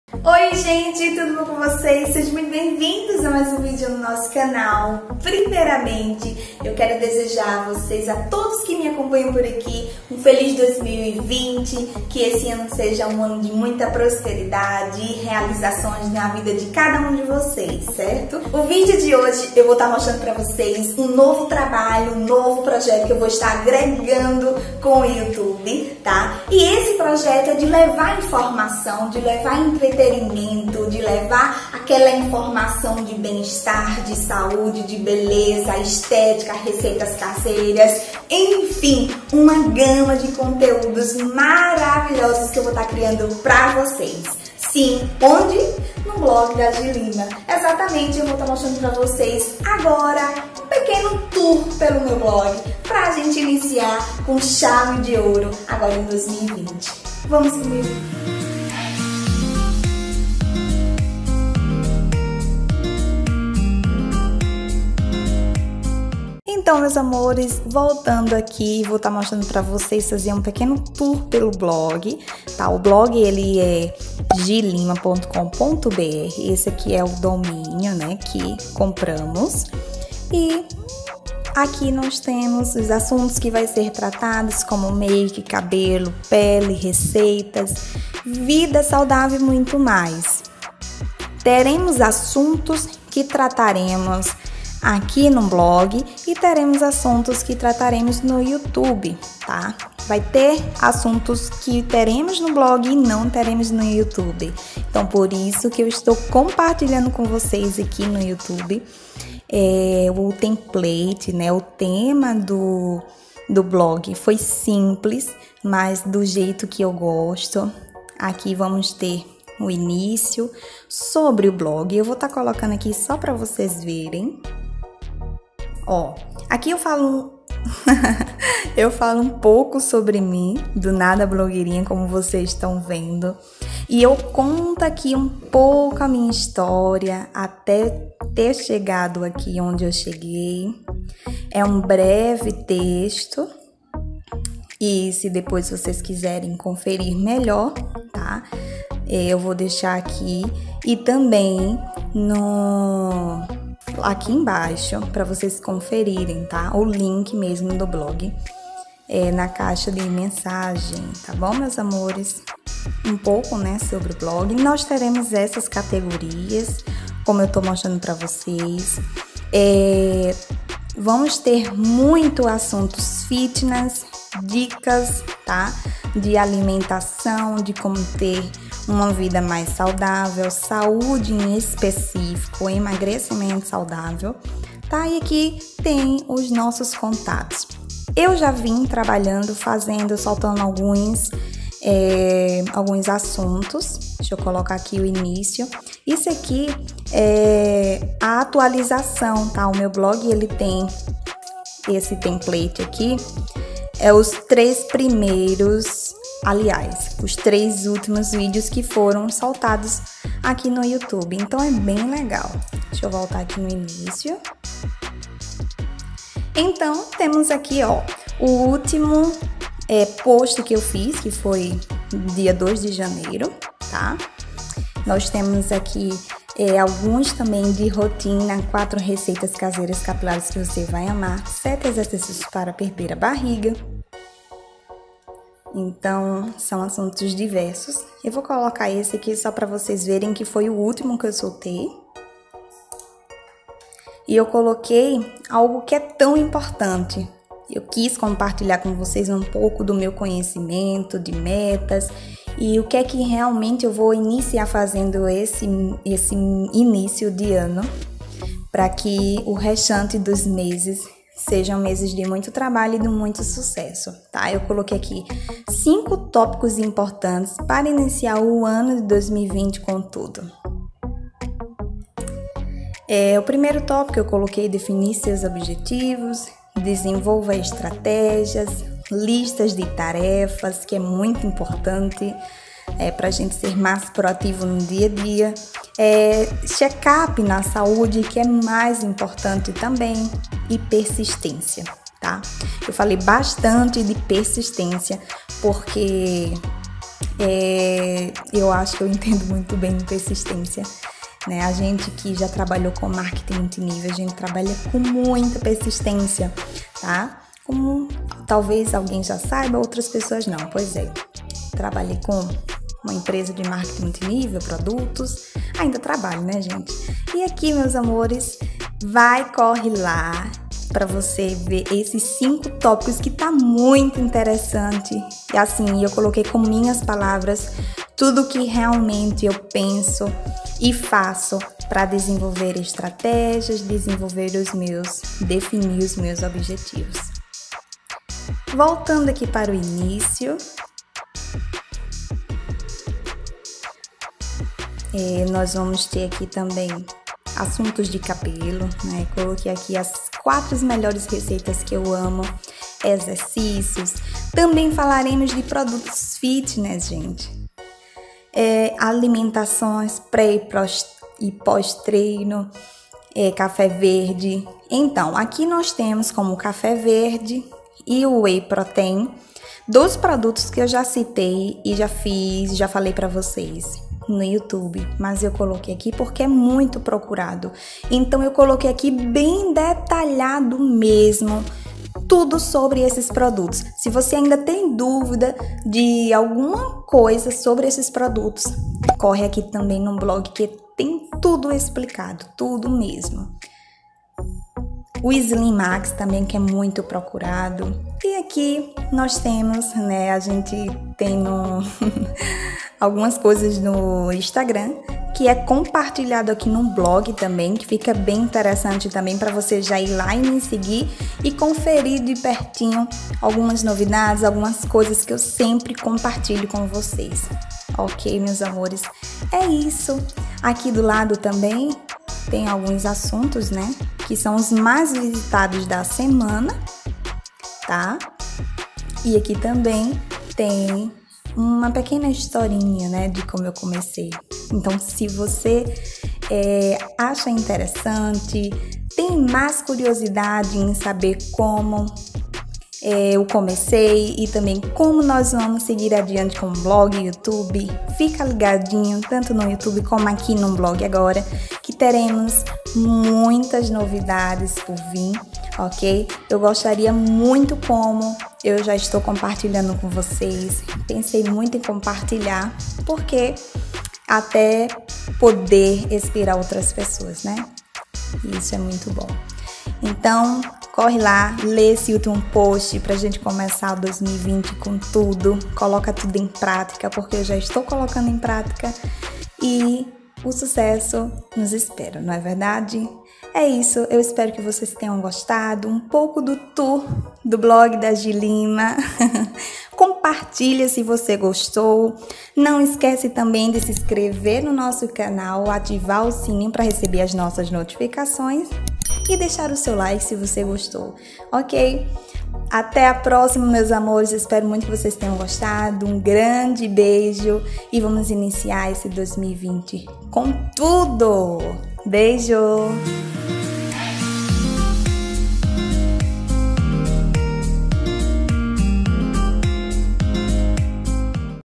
NOVIDADE PARA 2020, TOUR PELO MEU BLOG! ♥
Oii gente, tudo bom com vocês?!! Meus amores resolvi compartilhar mais um projeto que inicio este ano de 2020 com vocês, na qual estarei trazendo conteúdos diversos e estarei compartilhando lá no blog Gihlima, um blog novo, mais tenho certeza que teremos muitas coisas boas por lá e terei o prazer de compartilhar um pouco sobre mim, mais também um pouco de cada coisa, empreendedorismo, vida saudável, alimentação, receitas e muito mais, então nos acompanhe por lá também será um prazer ter sua visita!
grande beijos para todos♥